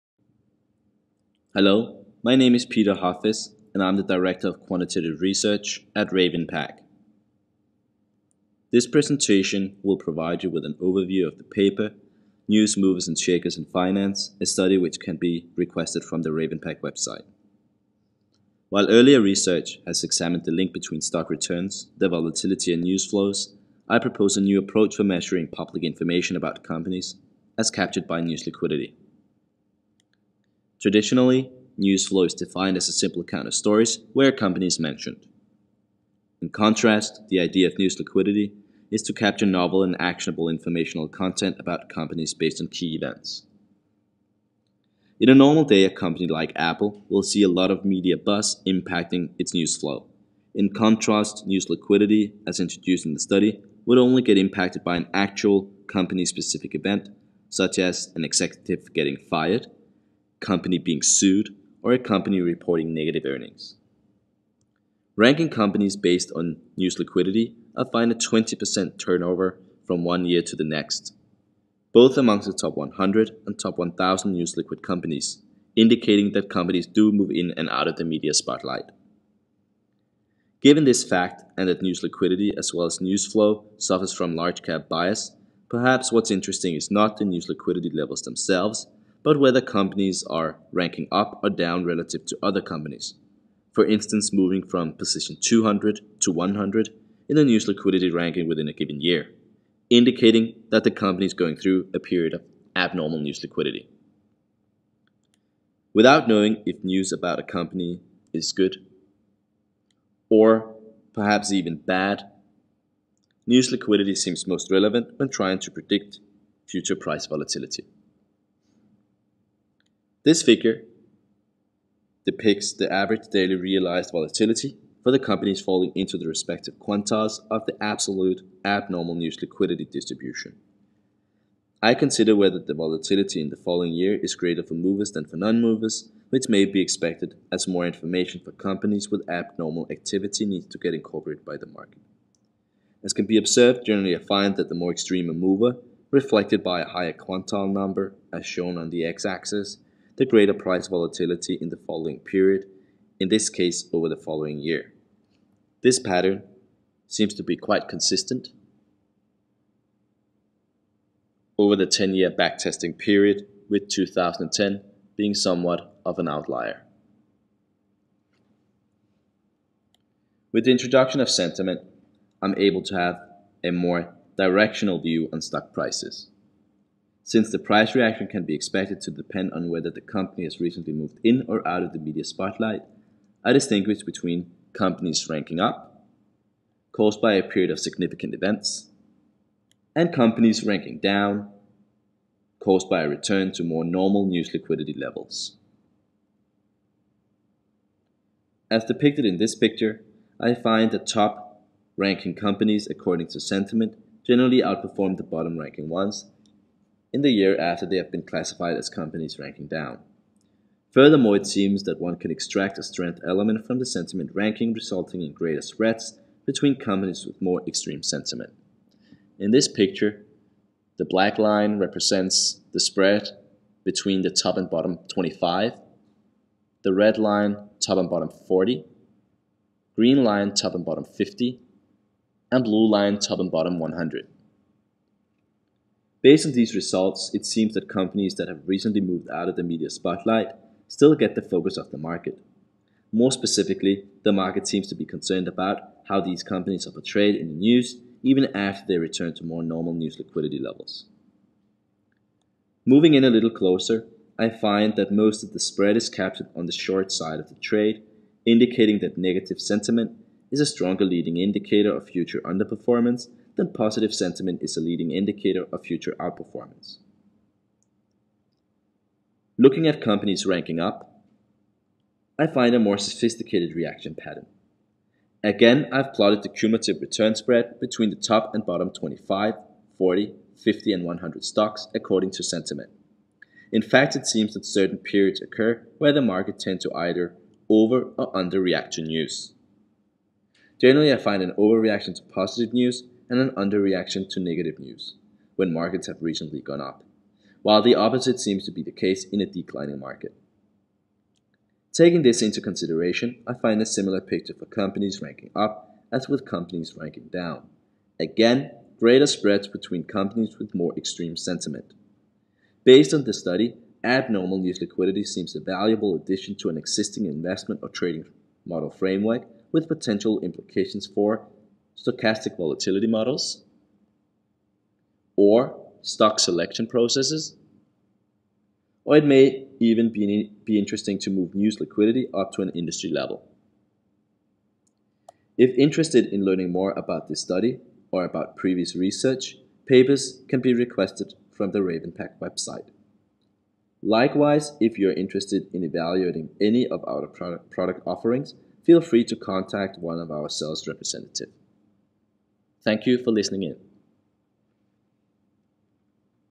How Media Coverage Impacts Market Performance
This study shows that an investor evaluating a universe of stocks should be concerned about tracking media coverage and sentiment.

The White Paper is available in PDF, you can download it